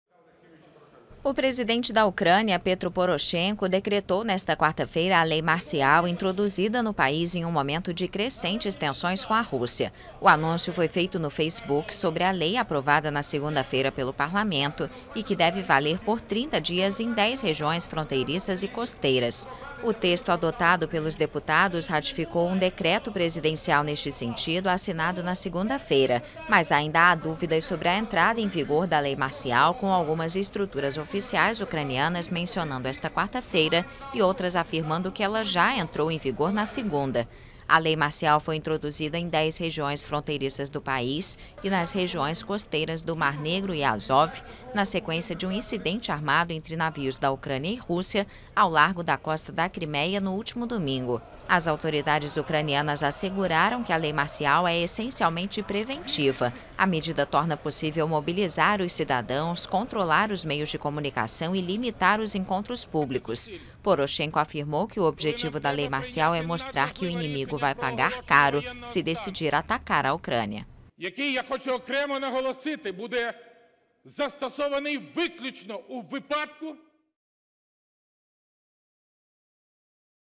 Presidente ucraniano promulga lei marcial
O presidente ucraniano, Petro Poroshenko, decretou nesta quarta-feira a lei marcial introduzida na Ucrânia num contexto de crescentes tensões com a Rússia o anúncio foi feito pelo porta-voz da presidência.